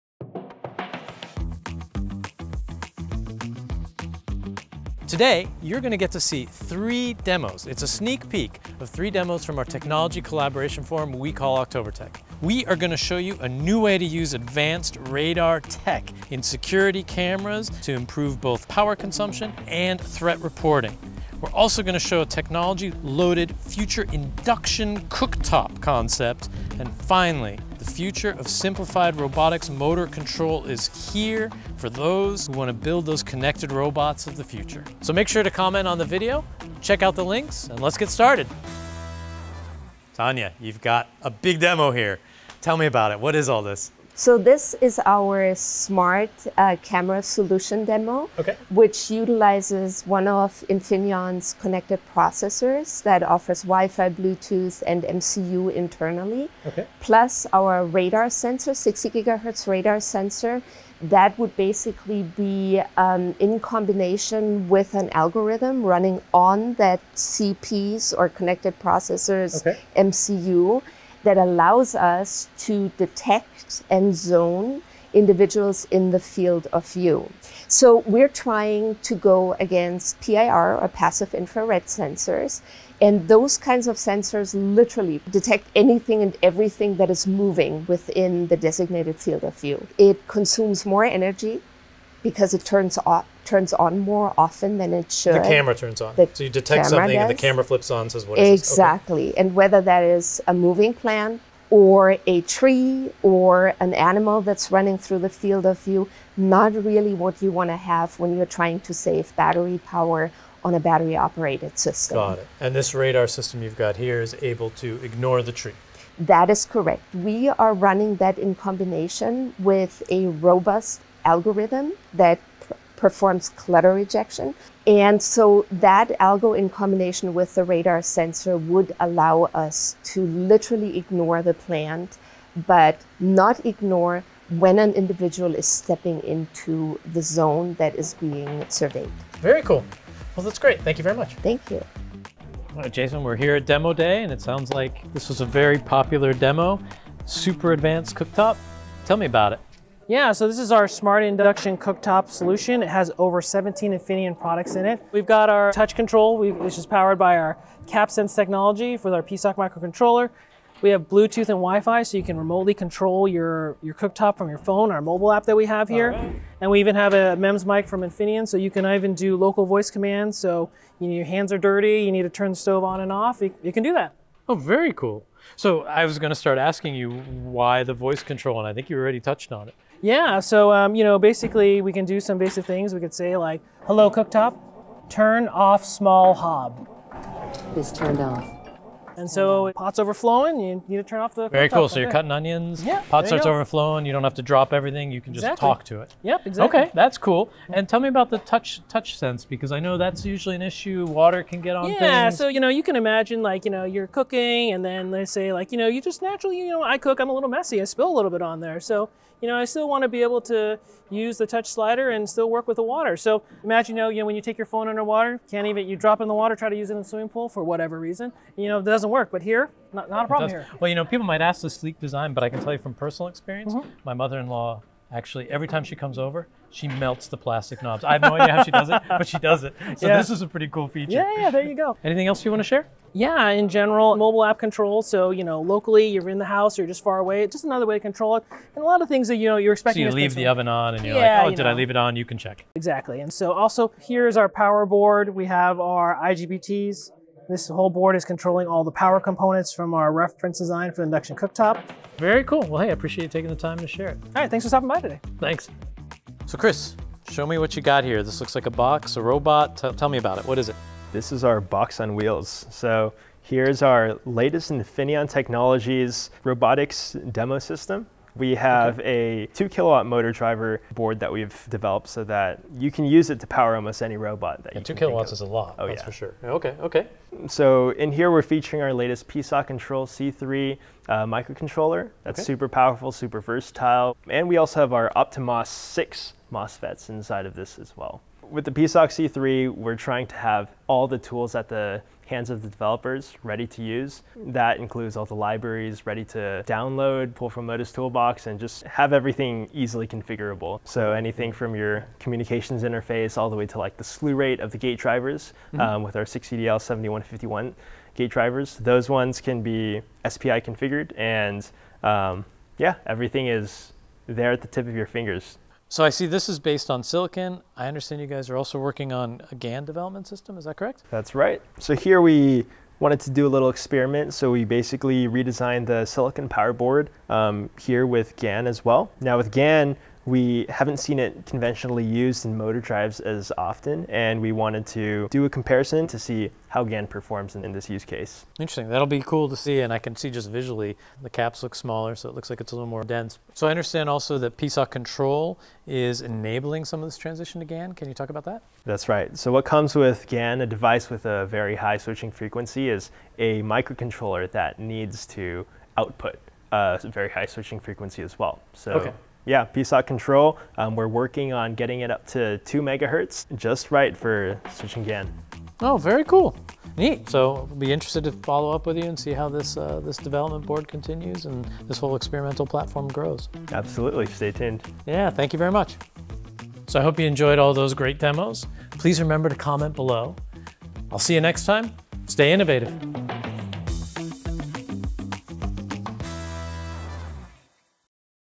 Innovation Insights: Inspiring solutions for IoT x3 | Infineon
Infineon's OktoberTech Silicon Valley showcased cutting-edge technology and the latest IoT innovations. If you missed it, you can still experience the excitement! Explore these captivating demos and be inspired for your next IoT breakthrough:

Invisible Radar: Witness the power of sensing without sight! See how this radar technology enhances security systems, detecting movement and identifying threats in new ways.

Smart Cooking: Experience the most intelligent and connected induction cooktop ever created, with intuitive controls, advanced safety, and seamless smart home integration.

Advanced Robotics: Bring your robotic visions to life! Learn how Infineon's Si, GaN, and PSOC Control C3M technologies empower faster, more agile robots, accelerating your journey from concept to reality.

Time stamps:
0:00-0:39 – Introduction:
0:40-2:21 – Smarter Wake-up using the XENSIV™ Radar Sensor in a Security Camera
2:22-4:14 – Smart Induction Cooktop
4:15-6:15 – 2 Kw Dual Motor Drive for Robotics using PSOC™ Control
6:16-6:28 – Closing